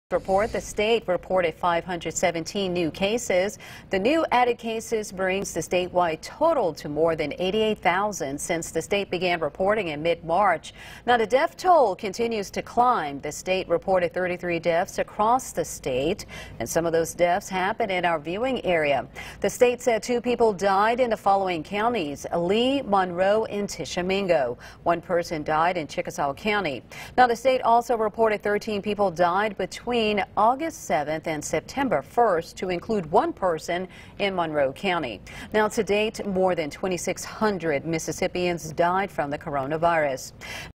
MSDH reports 517 new coronavirus cases, 33 new deaths Thursday
The Mississippi State Department of Health (MSDH) reported 517 new COVID-19 cases and 33 new deaths Thursday, Sept. 10.